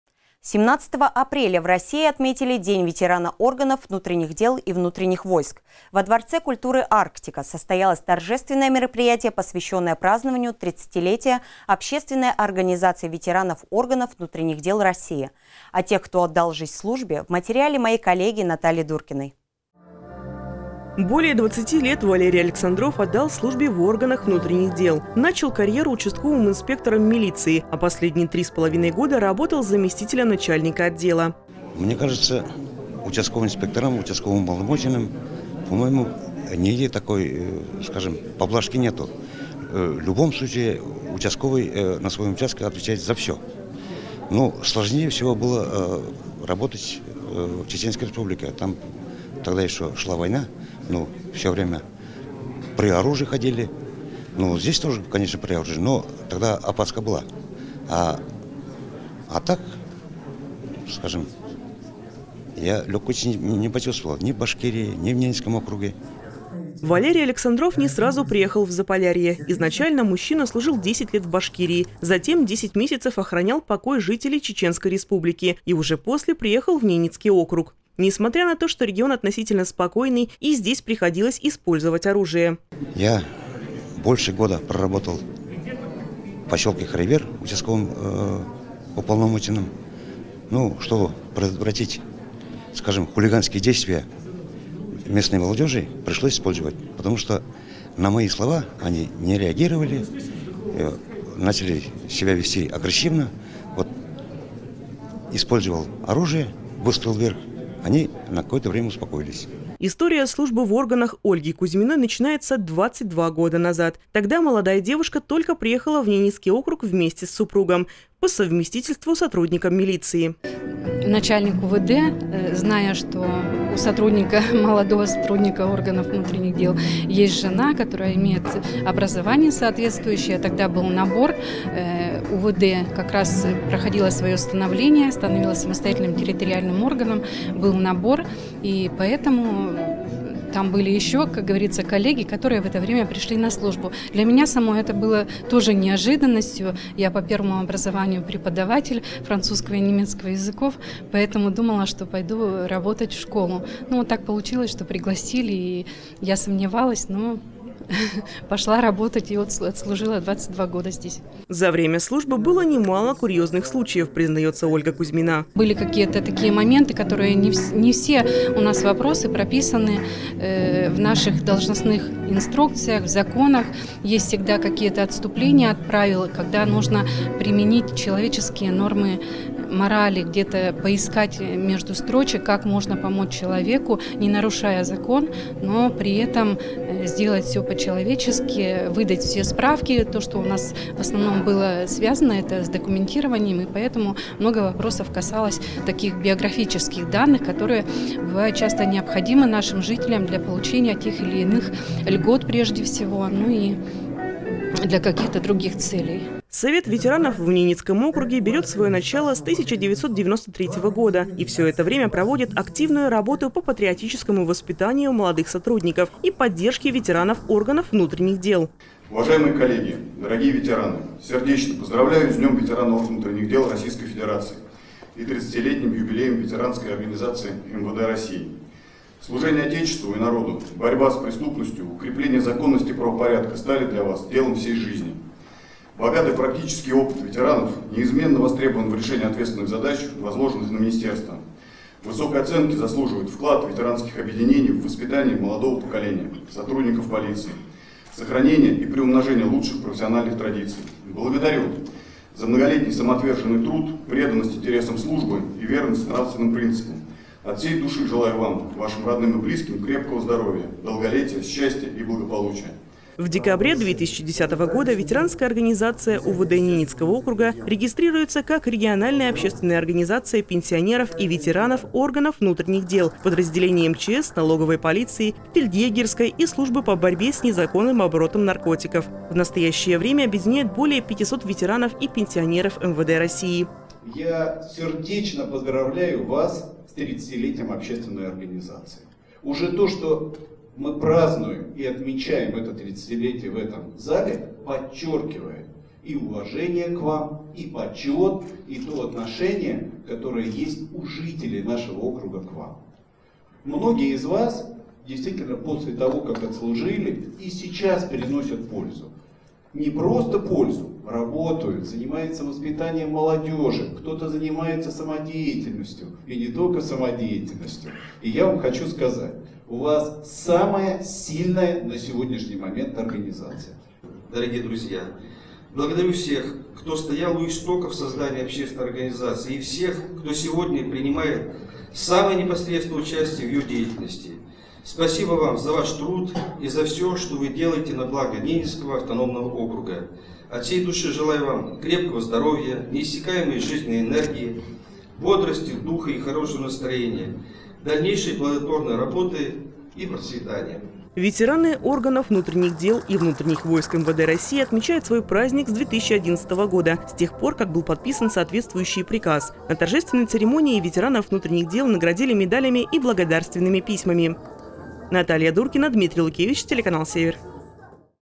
ДНИ И НОЧИ СЛУЖА ОТЕЧЕСТВУ
17 апреля в России отметили День ветерана органов внутренних дел и внутренних войск. Во дворце культуры Арктика состоялось торжественное мероприятие, посвященное празднованию 30-летия Общественной организации ветеранов органов внутренних дел России. О тех, кто отдал жизнь службе, в материале Натальи Дуркиной.